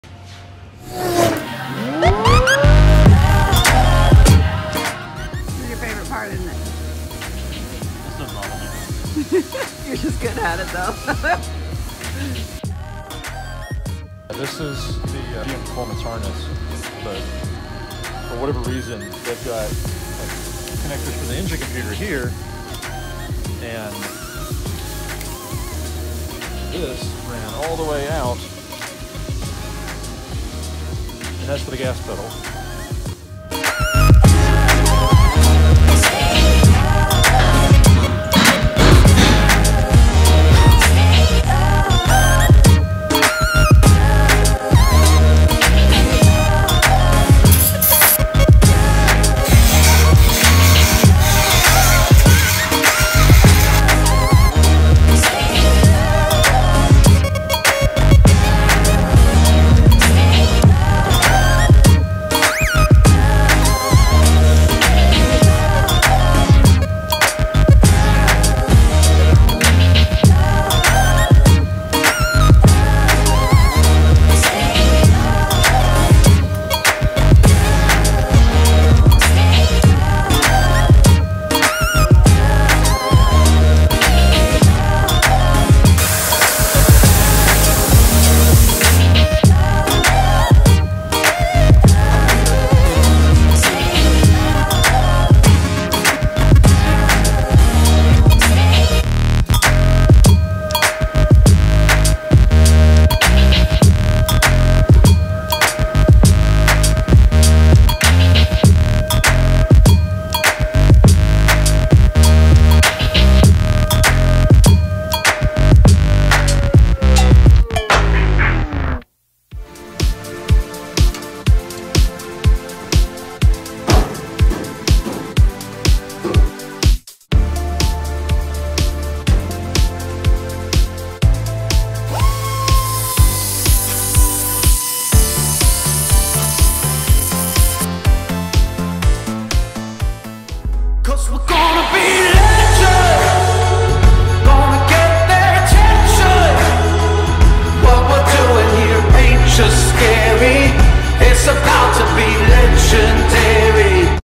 12/05/19 Wiring, Metal Working, and Bed Mock Up
Today we worked on the ‘58 GMC pickup, moving right along with the metal work and bodywork. We started wiring the ‘68 Chevy C10, and well that’s what we did all day- pretty much 🤪

What do you think of our daily videos?
Please leave your feedback below 🙏